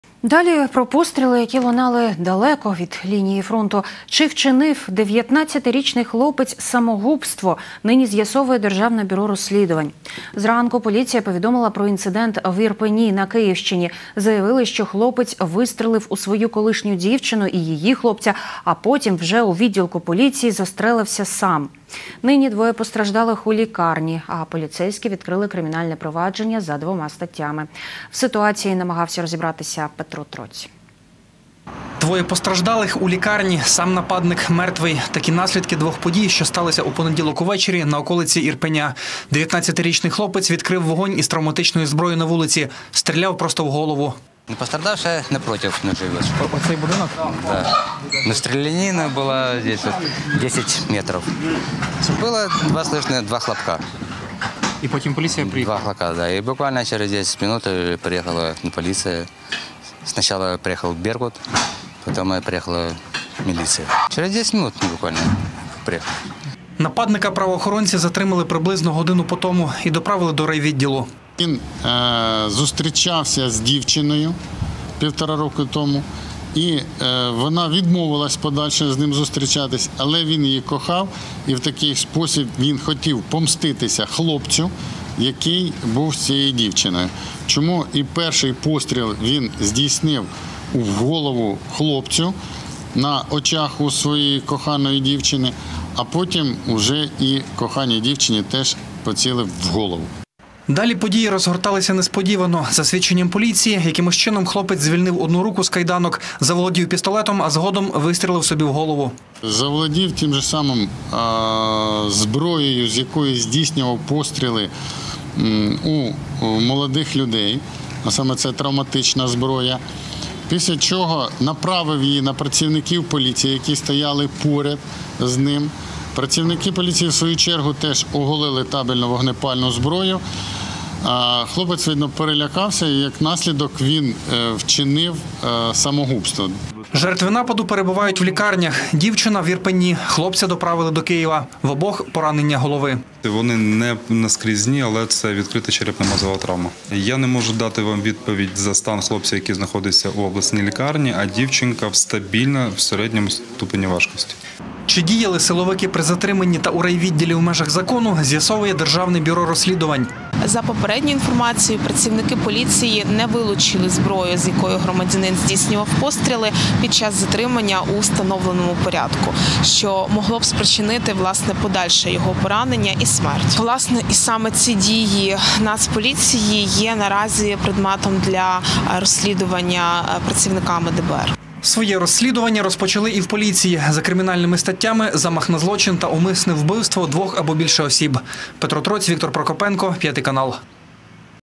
Стріляв у свою ексдівчину та її хлопця, а потім застрелився сам: подробиці НП в Ірпені
Чи вчинив 19-річний хлопець самогубство, нині з'ясовує Державне бюро розслідувань. Зранку поліція повідомила про інцидент в ІрпенІ на Київщині. Заявили, що хлопець вистрілив у свою колишню дівчину і її хлопця, а потім вже у відділку поліції застрелився сам. Нині двоє постраждалих у лікарні. А поліцейські відкрили кримінальне провадження за двома статтями.